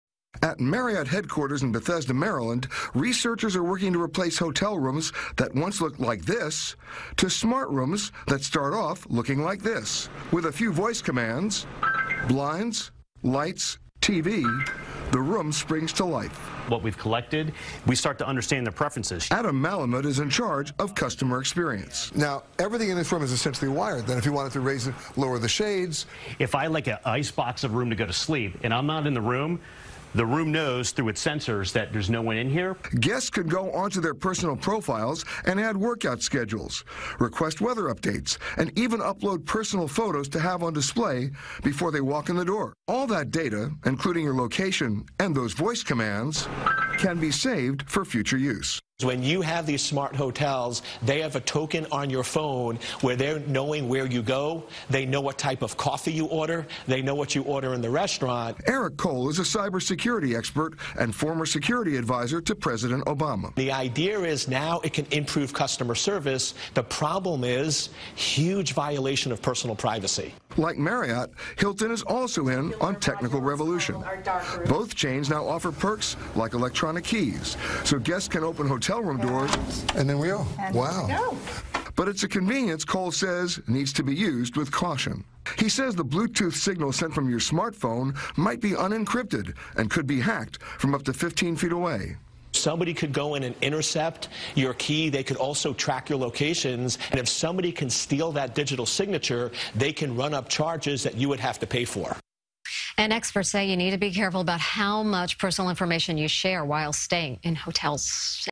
Smart Hotels? Digital Makeovers Coming
Hotels are giving their rooms digital makeovers, but CBS2's Peter Greenberg says customers need to be mindful of their personal information.